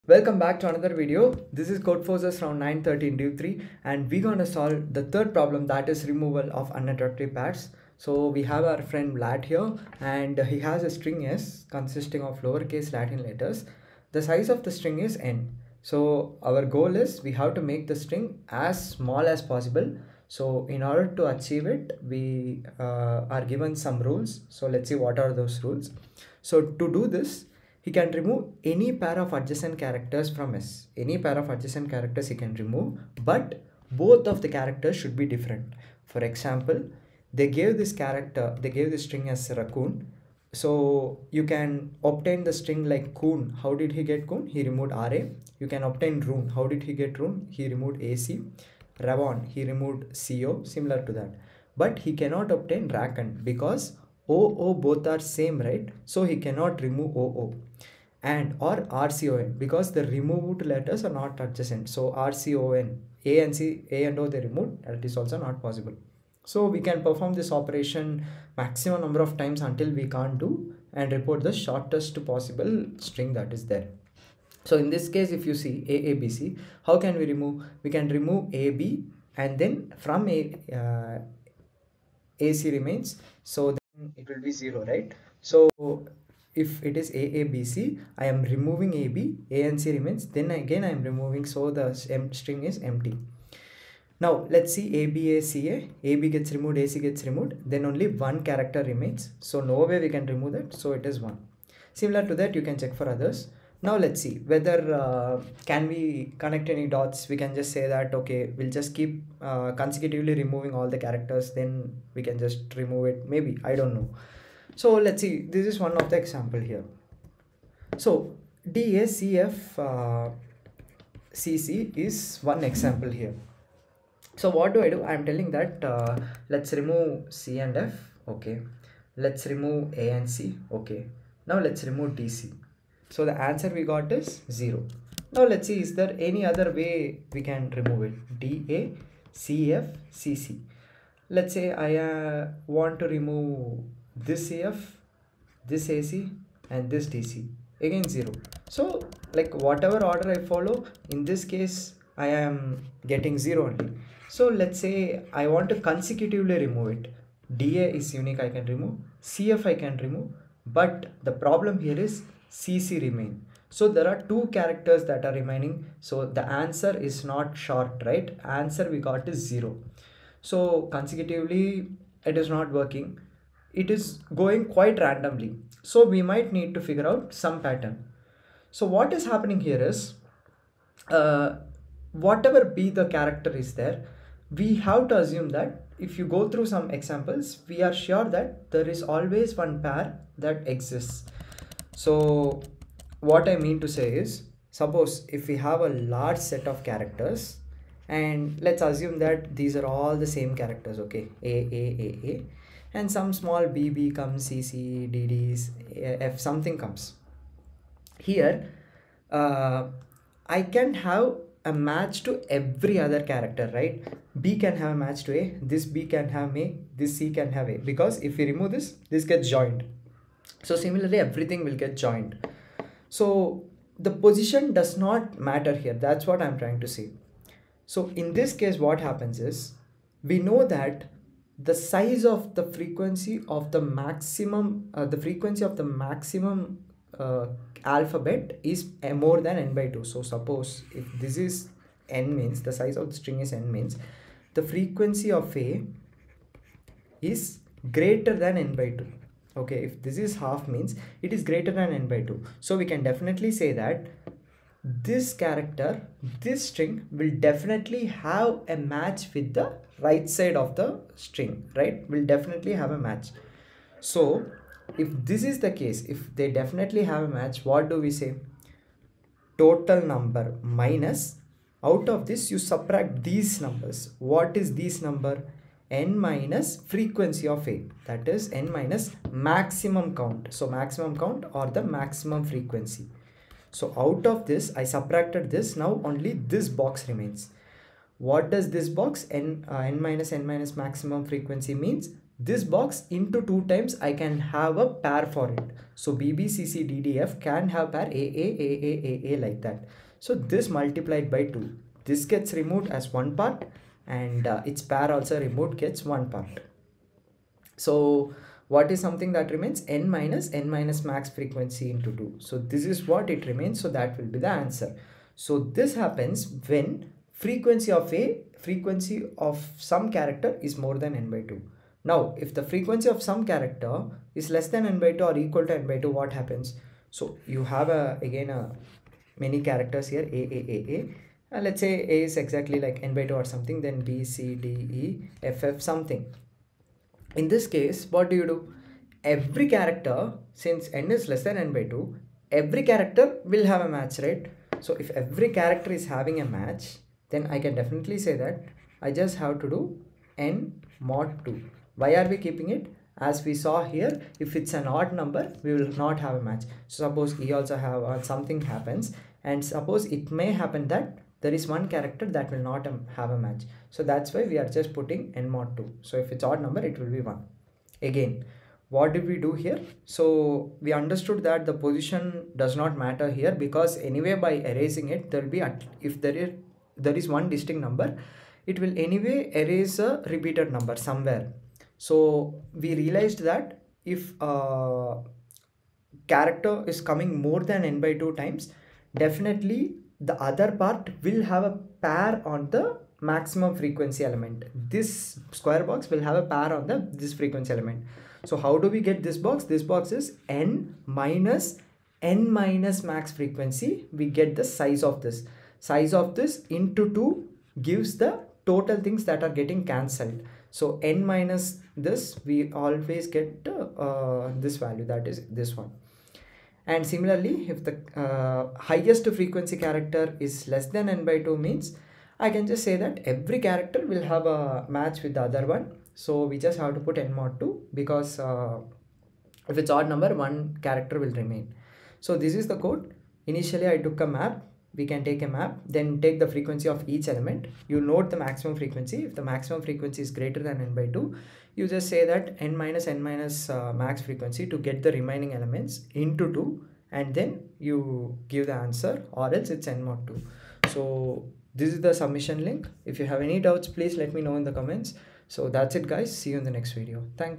Removal of Unattractive Pairs | Codeforces 1907C Solution | Codeforces Round 913 (Div. 3) | English
In this video, I walk through the solution to problem C from Codeforces Round 913 (Div. 3)

Problem name: "removal of unattractive pairs"
My Submission:
I explain the thought process and logic behind the solution and show how to implement it in code.